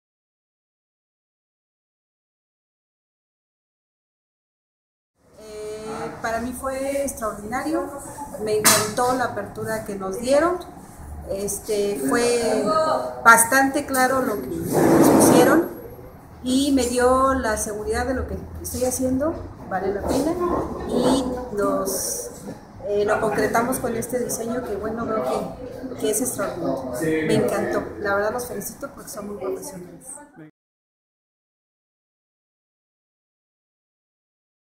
Cursos de jardines verticales en Guadalajara, México
Opinión de los asistentes a los cursos de jardines verticales de México, en este caso, de Guadalajara.